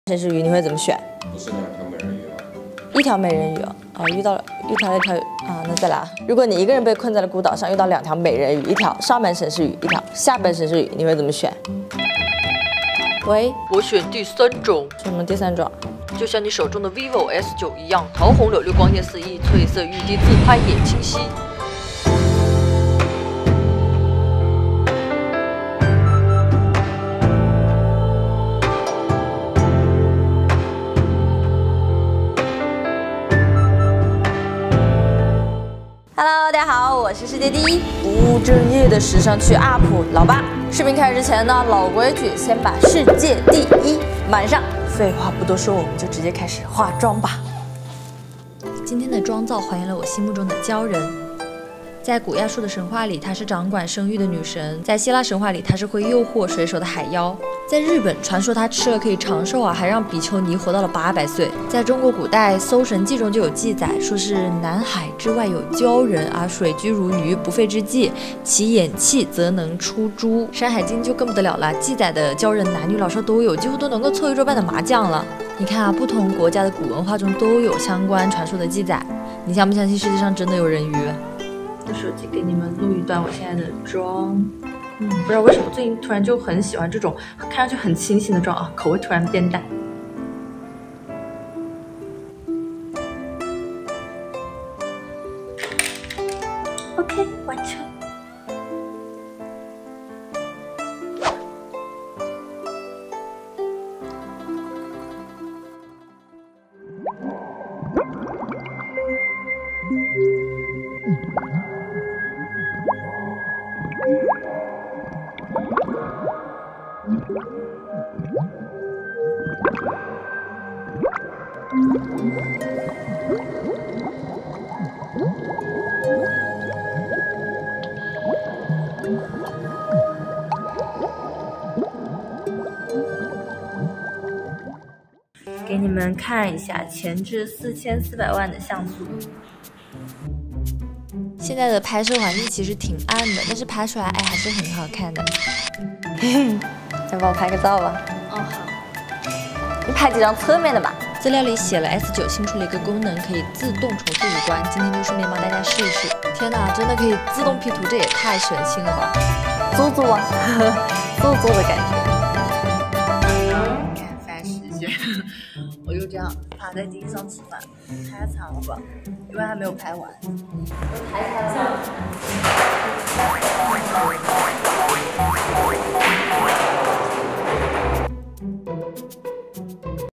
【八】鲛人真的存在？打造“泣珠妆”秒变美人鱼
不相信有鲛人的点赞，相信她存在的就三连吧！

饰品来自：蝉鸣知夏设计工作室【凌波仙】
摄影@摄影师白木 发型@小狐狸 剪辑@卢酱@老八捌